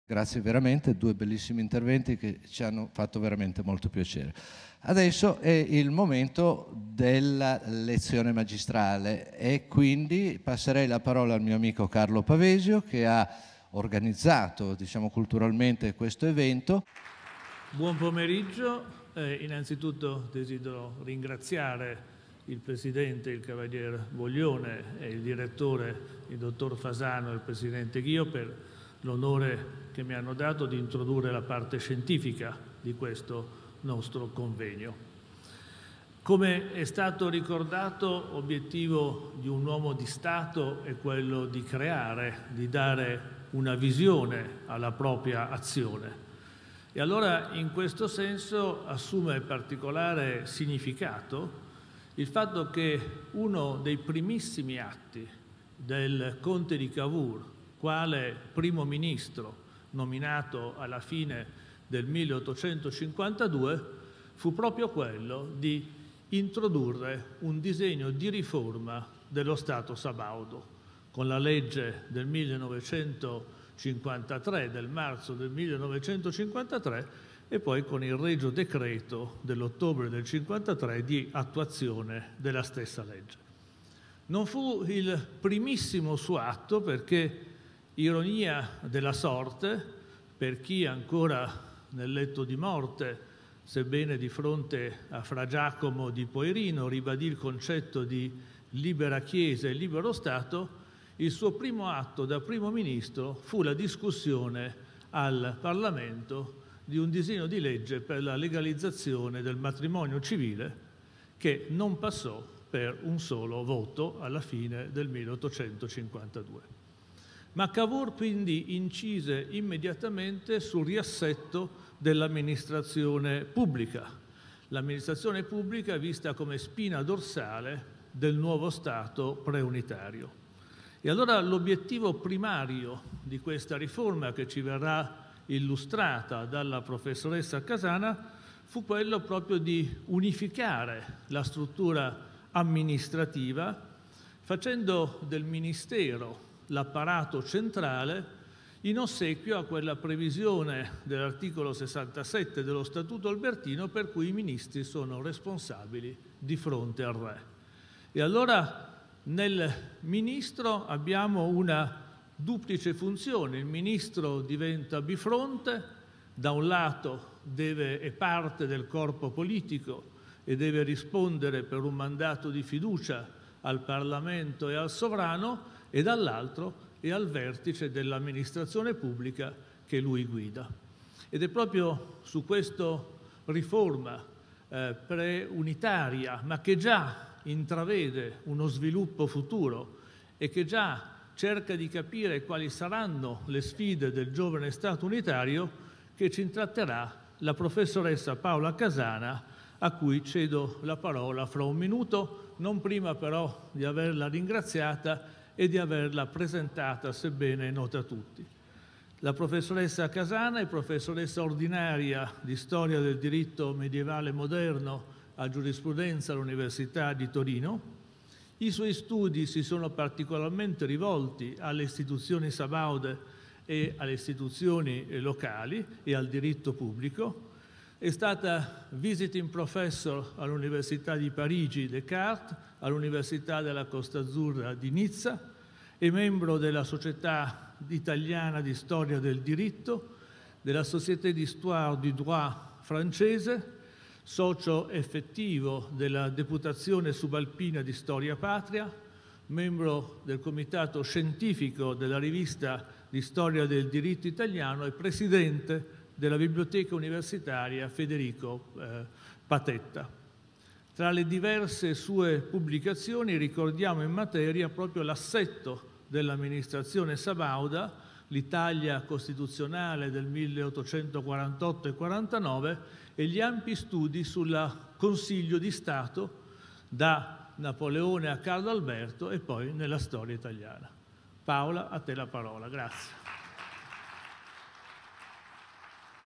Carlo Pavesio, delegato alle attività scientifiche e culturali della Fondazione Cavour.
L’avvocato Pavesio introduce l’intervento della professoressa Paola Casana al secondo convegno del Centro Alti Studi per lo Stato promosso e organizzato dalla Fondazione Cavour con il sostegno della Compagnia di San Paolo.